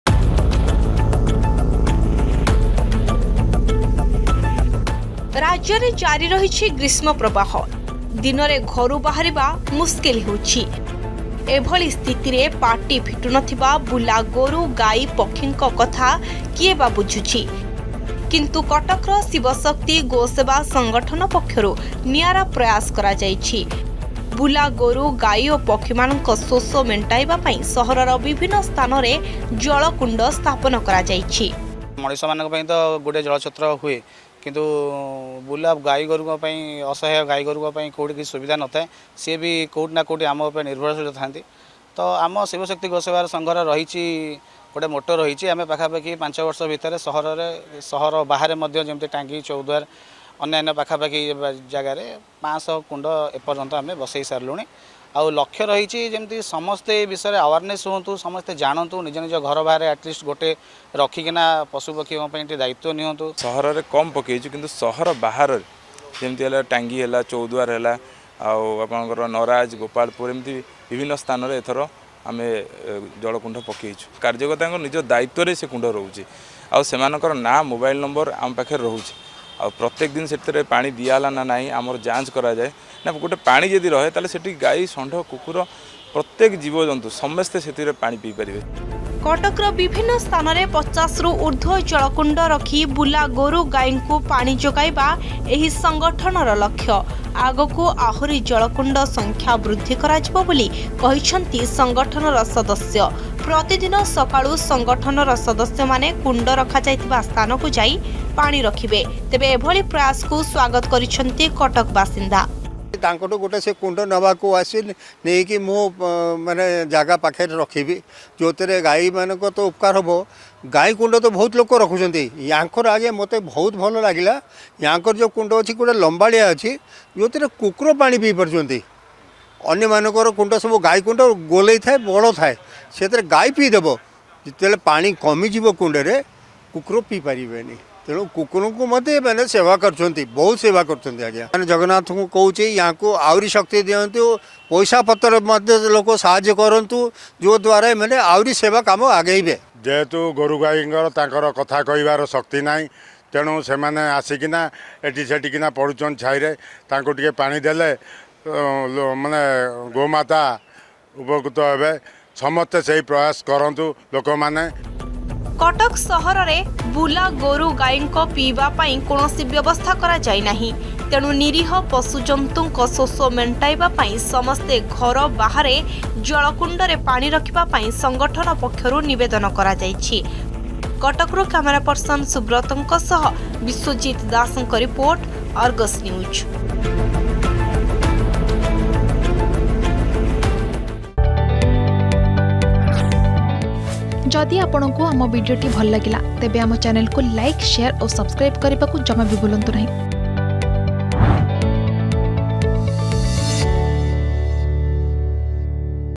ବୁଲା ଗୋରୁ ଗାଇ ଓ ପକ୍ଷୀ ମାନଙ୍କ ଶୋଷ ମେଣ୍ଟାଉଛନ୍ତି କିଛି ଯୁବ ଗୋଷ୍ଠୀ || Go Seva Organization
ରାଜ୍ୟରେ ଜାରି ରହିଛି ଗ୍ରୀଷ୍ମପ୍ରବାହ । ଦିନ ୧୦ଟା ପରେ ଘରୁ ବାହାରିବା ଏକ ପ୍ରକାର ମୁସ୍କିଲ ହେଉଛି । ଏହାରି ଭିତରେ ବୁଲା ଗୋରୁ ଗାଇଙ୍କ ଶୋଷ ମେଂଟେଇବା ପାଇଁ ଭିନ୍ନ ଏକ ପ୍ରୟାସ ଆରମ୍ଭ କରିଛନ୍ତି କଟକ ସହରର କିଛି ଯୁବ ଗୋଷ୍ଠୀ । ସହରର ବିଭିନ୍ନ ସ୍ଥାନରେ ଜଳ କୁଣ୍ଡ ରଖି ନିରୀହ ପଶୁ ପକ୍ଷୀ ମାନଙ୍କୁ ପିଇବା ପାଣି ଯୋଗାଉଛନ୍ତି  ଶିବଶକ୍ତି ଗୋସେବା ସଂଗଠନର କର୍ମୀ ।